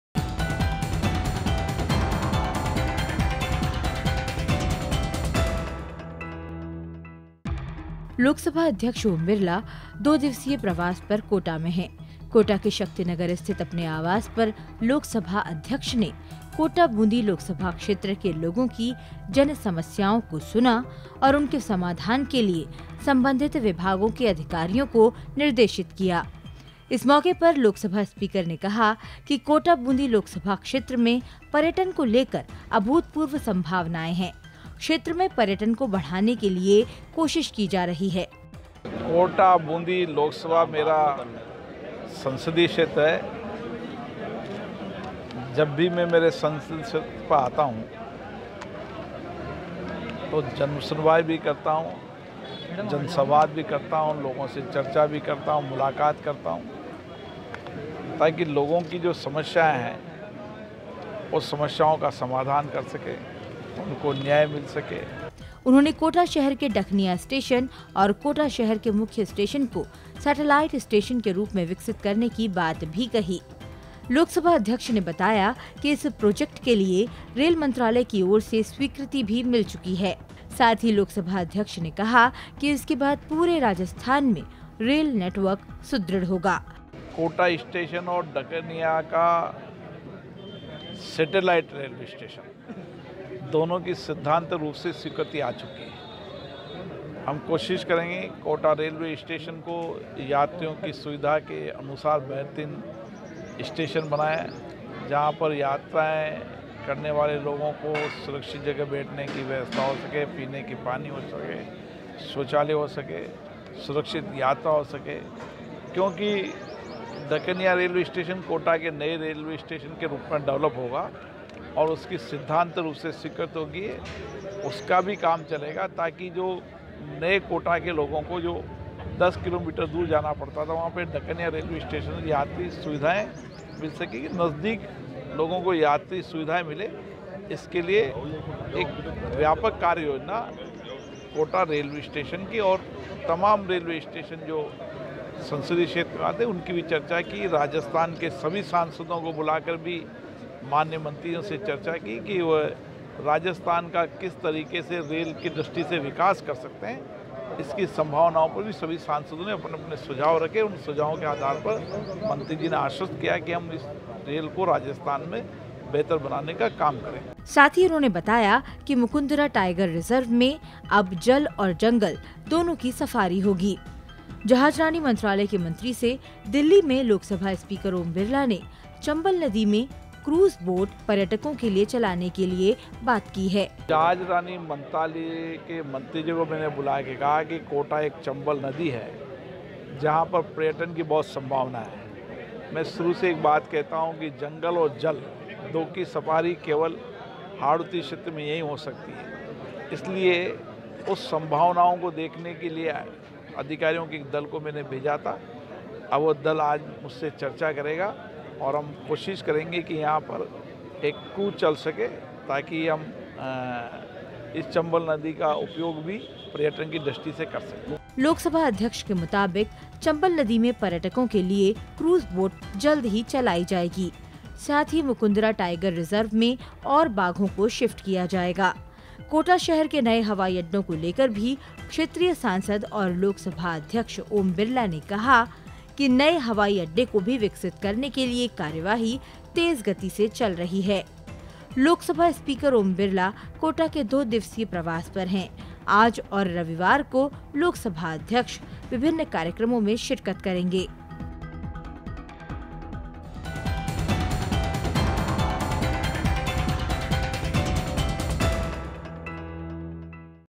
कोटा में जनसुनवाई- 15 फरवरी, 2020
अपने संसदीय क्षेत्र कोटा के दो दिवसीय दौरे के दौरान अपने आवास पर जनसुनवाई की. जनसमस्याओं के समाधान के लिए संबंधित विभागों को निर्देशित किया. विभिन योजनाओं के क्रियान्वयन को लेकर मीडिया के साथ जानकारी भी साझा की.